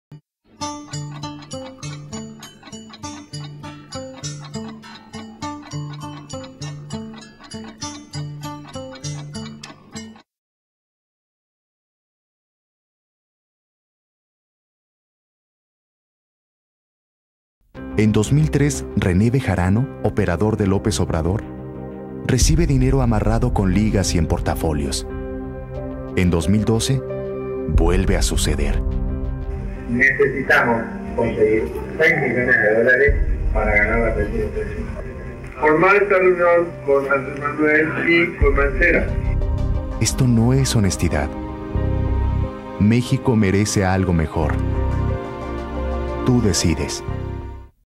Enrique Peña Nieto "Charolazo"
El Instituto Federal Electoral dio a conocer las pautas de los candidatos a la Presidencia de México: Andrés Manuel López Obrador, Enrique Peña Nieto, Josefina Vázquez Mota y Gabriel Quadri